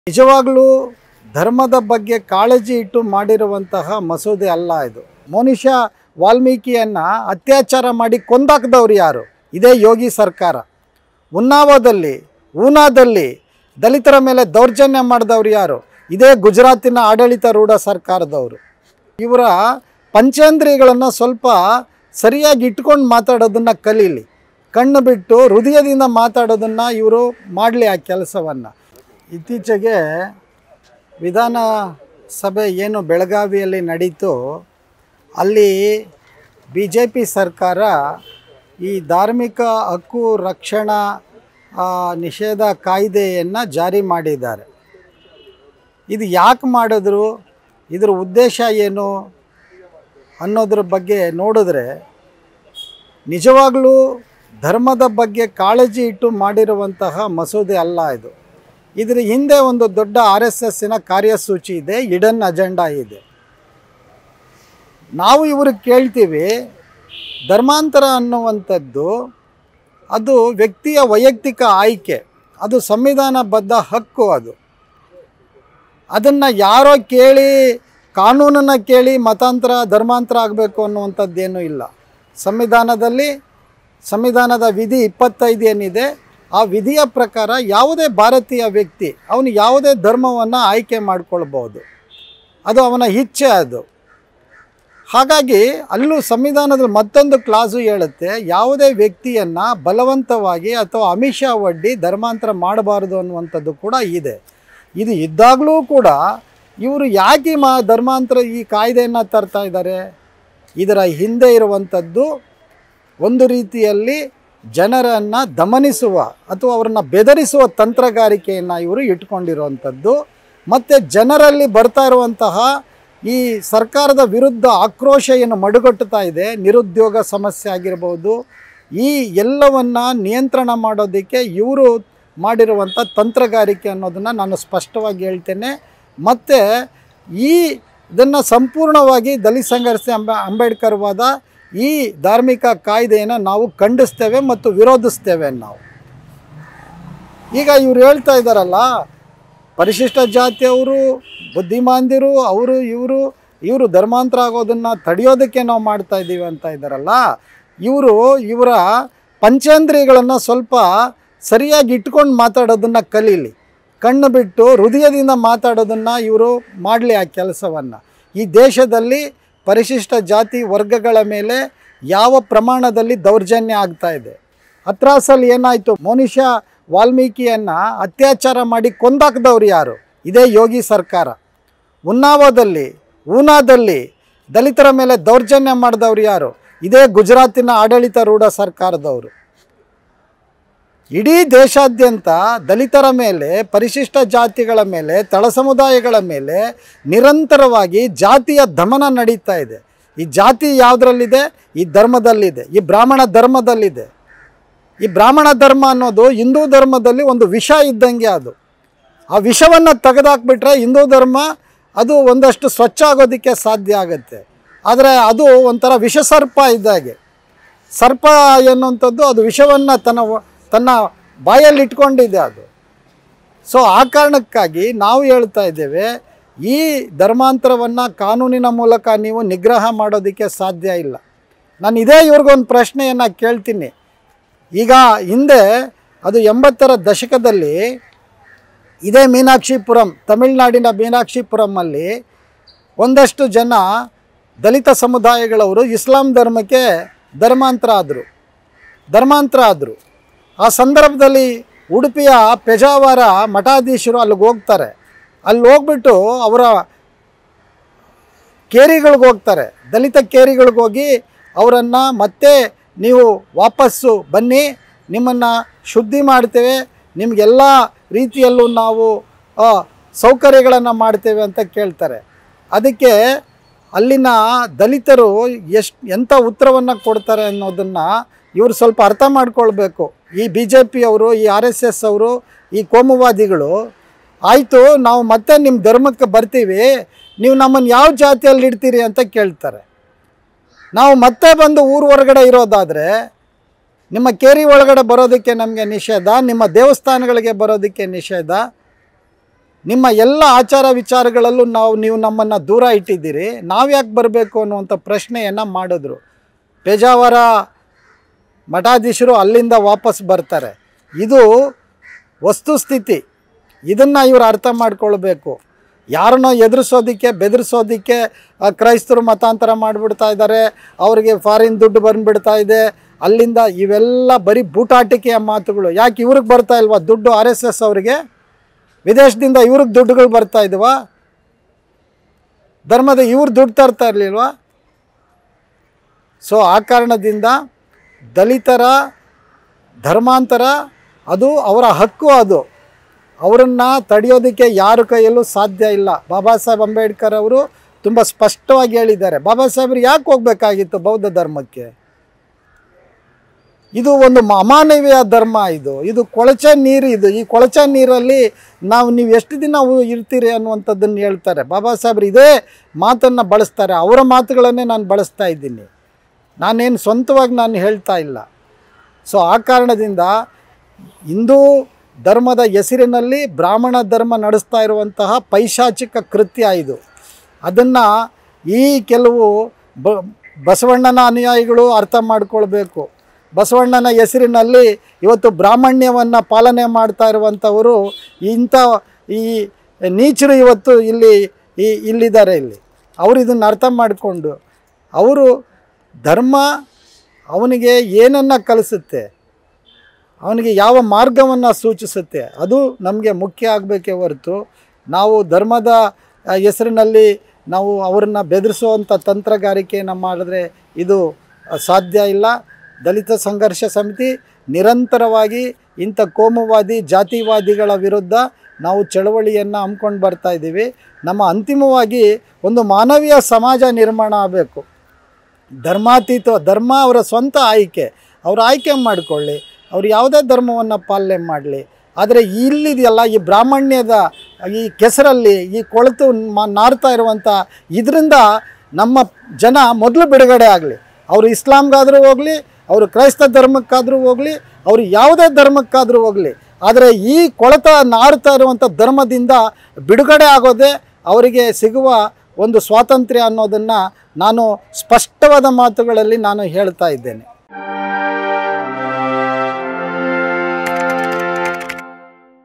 ''ಮತಾಂತರ ನಿಷೇಧ ಕಾಯಿದೆಯ ಹಿಂದೆ ಆರೆಸ್ಸೆಸ್ ಕಾರ್ಯಸೂಚಿ '': ಮಾವಳ್ಳಿ ಶಂಕರ್
► ಬ್ರಾಹ್ಮಣ್ಯದ ವಿಷವನ್ನು ಹಿಂದೂ ಧರ್ಮದಿಂದ ತೆಗೆಯಬೇಕು: ಮಾವಳ್ಳಿ

► ''ಧರ್ಮಾಂತರ ವ್ಯಕ್ತಿಯ ವೈಯಕ್ತಿಯ ಆಯ್ಕೆ, ಸಂವಿಧಾನ ಬದ್ಧ ಹಕ್ಕು''

► ''ಸರಕಾರ ಕಣ್ಣು ಬಿಟ್ಟು ಹೃದಯದಿಂದ ಮಾತನಾಡಲು ಕಲಿಯಲಿ''